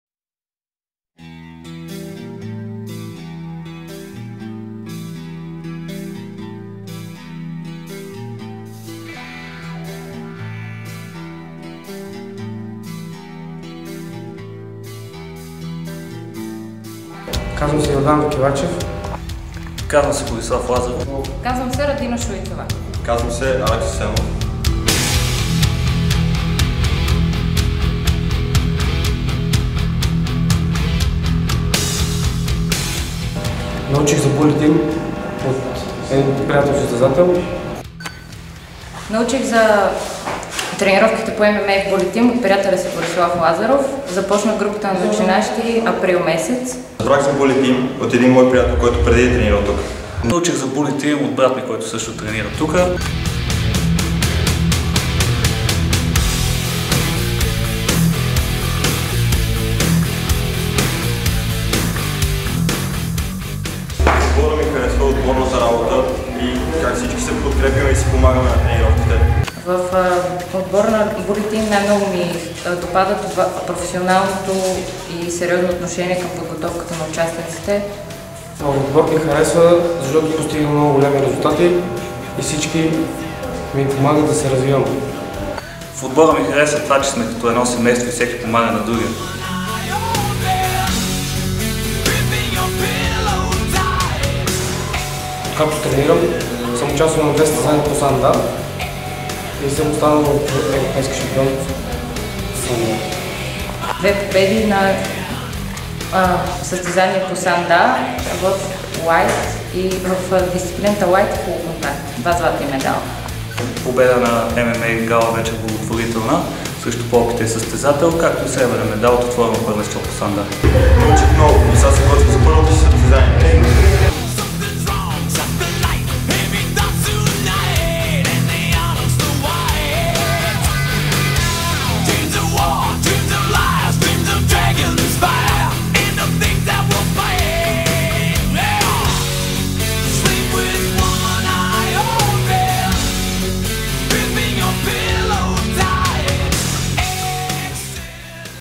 FC Bully Team Starts New Beginners Group
На 6 Януари 2014 Були Тийм стартира нова група за начинаещи. Всеки, който иска да направи своите първи стъпки в ММА е поканен! Мястото е бул. Черни връх 47, ет.2 (бивш завод Витоша), за повече информация посетете страницата ни във Фейсбук: